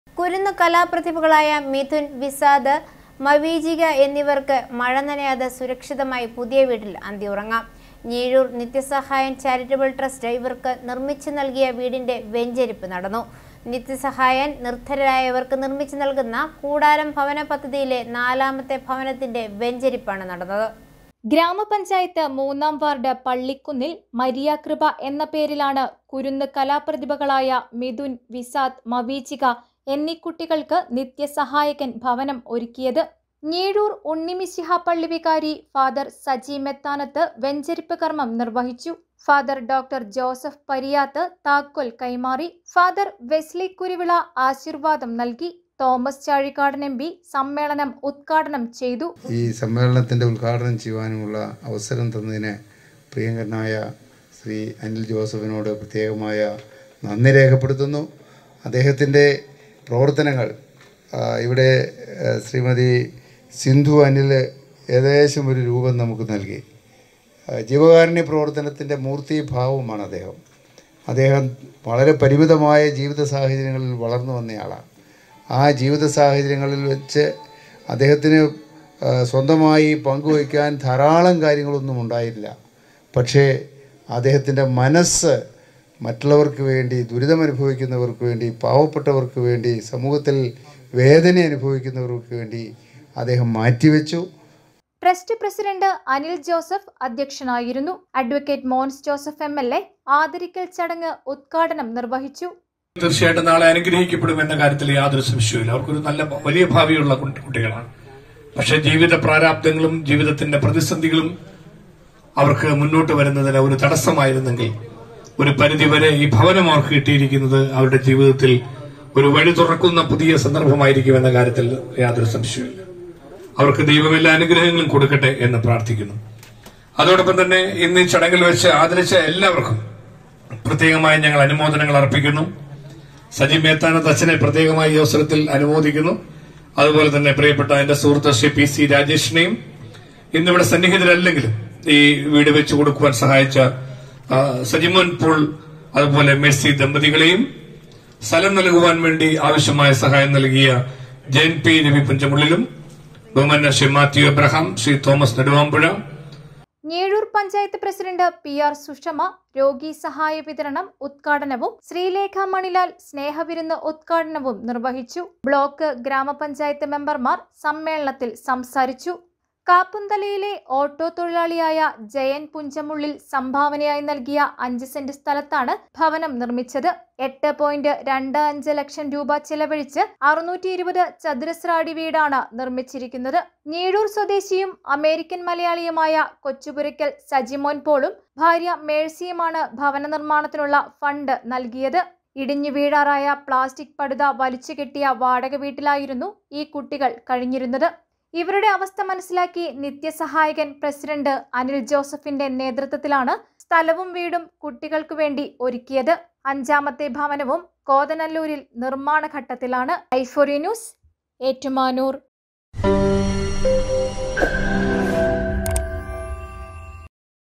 കൂടാരം ഭവന പദ്ധതിയിലെ നാലാമത്തെ ഭവനത്തിന്റെ വെഞ്ചരിപ്പ് നടന്നു
കുരുന്ന് കലാപ്രതിഭകളായ മിഥുന്‍, വിസാദ്,  മവീജിക എന്നിവര്‍ക്ക് മഴ നനയാതെ സുരക്ഷിതമായി പുതിയ വീട്ടില്‍ അന്തിയുറങ്ങാം. ഞീഴൂര്‍ നിത്യസഹായന്‍ ചാരിറ്റബിള്‍ ട്രസ്റ്റ് ഇവര്‍ക്ക് നിര്‍മ്മിച്ചു നല്‍കിയ  വീടിന്റെ വെഞ്ചിരിപ്പ് നടന്നു. നിത്യസഹായകന്‍ നിര്‍ധനരായവര്‍ക് നിര്‍മ്മിച്ചു നല്‍കുന്ന കൂടാരം ഭവന പദ്ധതിയിലെ നാലാമത്തെ ഭവനത്തിന്റെ വെഞ്ചരിപ്പാണ് നടന്നത്